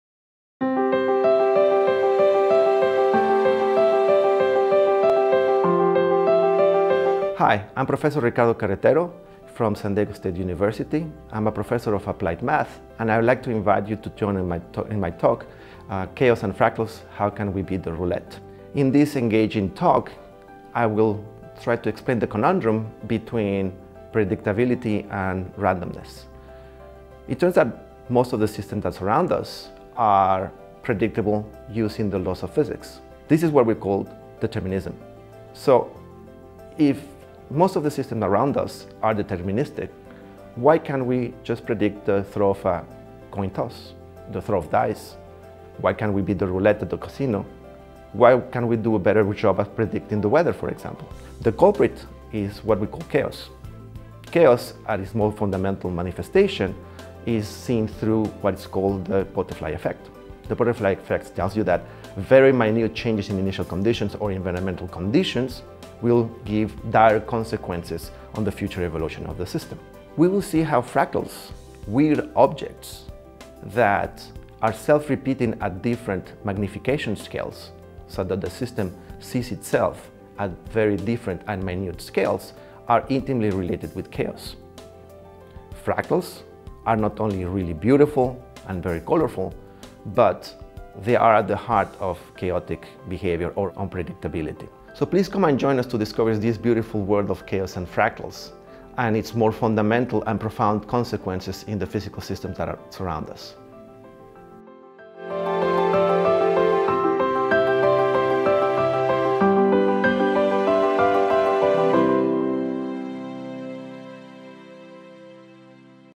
Chaos and Fractals: Why Can't We Beat the Roulette? With Ricardo Carretero
Explore the conundrum between determinism and randomness through the concept of chaos and its beautiful fractal signatures. The laws of physics dictate that if one knows all forces acting on a system, then its future evolution is strictly determined by its initial configuration. Yet why do certain systems display apparently unpredictable behavior? For example, is a coin toss a random event? Can we predict the roulette? The topic is fascinating.